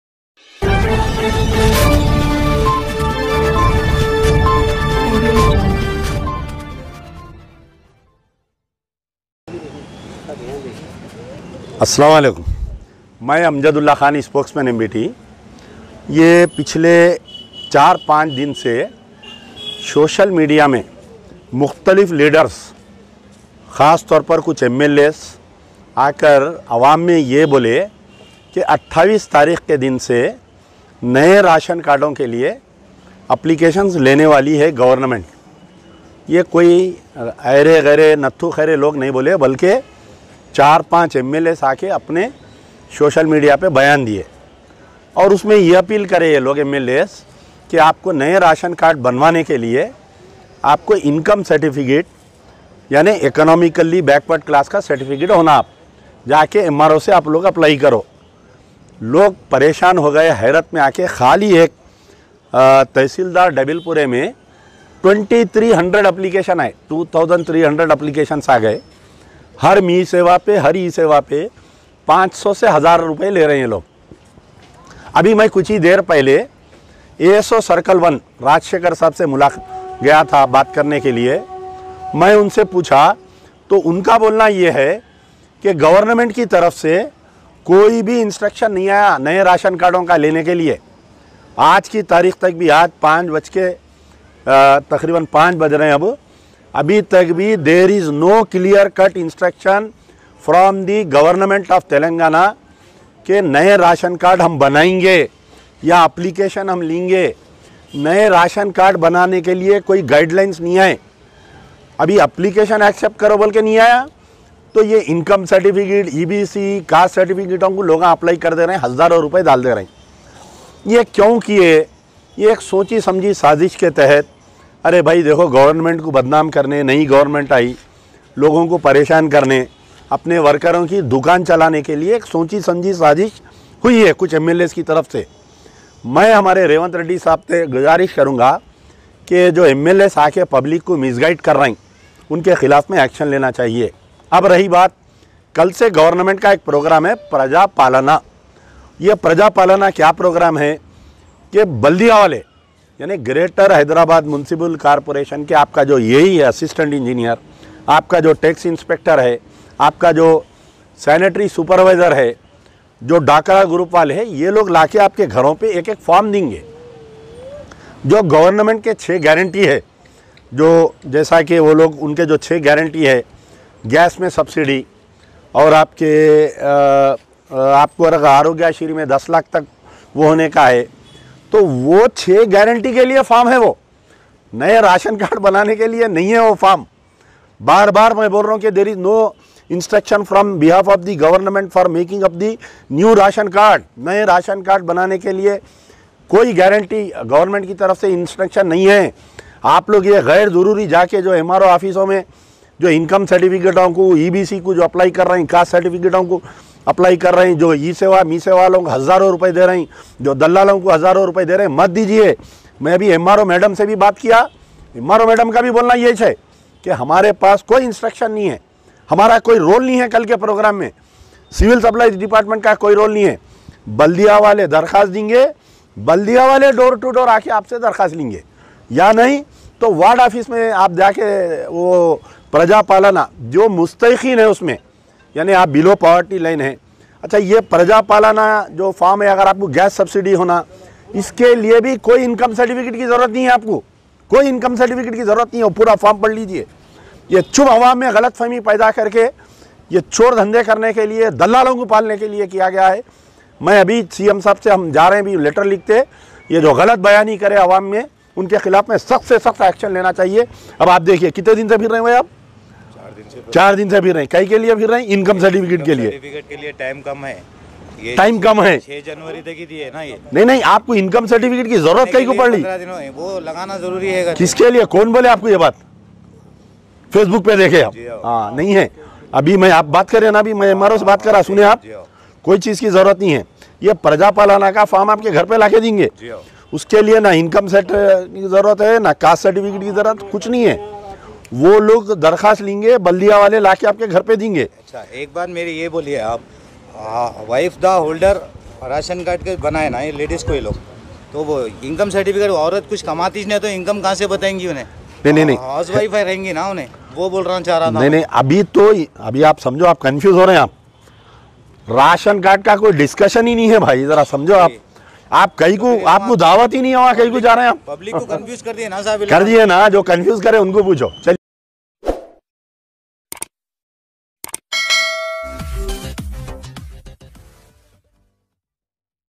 News53🖋️NEW RATION CARDS AUR INCOME CERTIFICATE KO LAYKAR AMJADULLAH KHAN MBT SPOKESPERSON NE KIYA
NEW RATION CARDS AUR INCOME CERTIFICATE KO LAYKAR AMJADULLAH KHAN MBT SPOKESPERSON NE KIYA KHULASA .AKHAR Q AWAM KO PARESHAN KIYA GAYA HAI MEESEVA AUR ESEWA PE AWAM KA EK HUJOOM.KOUN HAI ISKE ZUMEDAR .